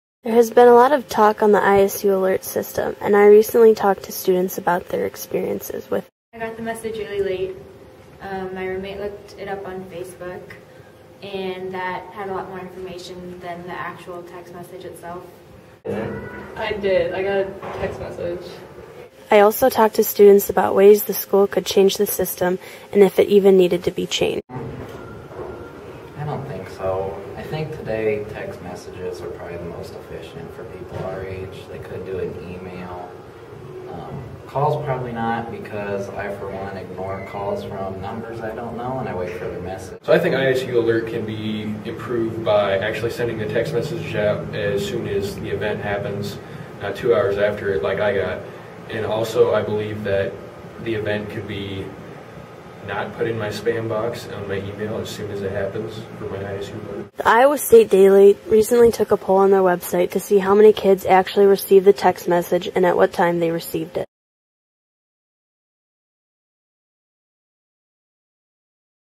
ISU Alert System
The ISU Alert System has been given flack by students recently.  Students were asked how they felt about the system and if it needed to be changed.